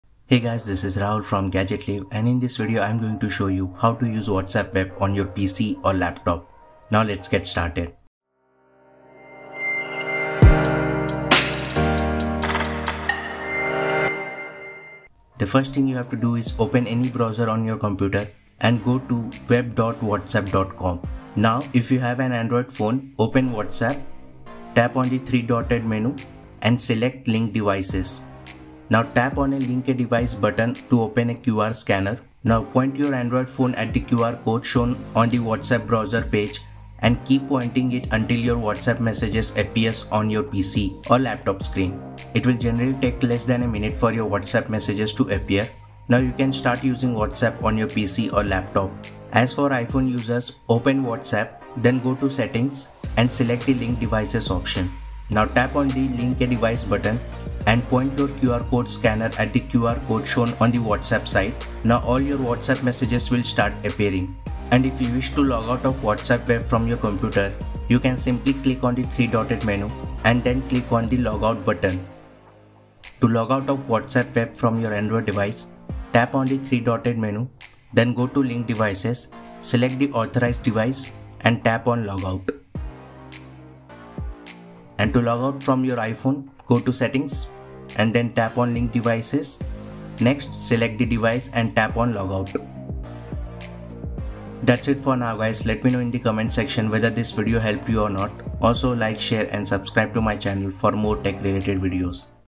How to Use WhatsApp Web on PC or Laptop (2022)
In this video, I'm going to show you how to use WhatsApp Web on your PC or Laptop in 2022.

Text version of this video

VIDEO TIMESTAMP

0:00 Intro
0:15 Use WhatsApp Web with Android
0:49 Use WhatsApp Web with iPhone
1:04 Log out of WhatsApp Web via PC
1:12 Log out of WhatsApp Web via Android
1:25 Log out of WhatsApp Web via iPhone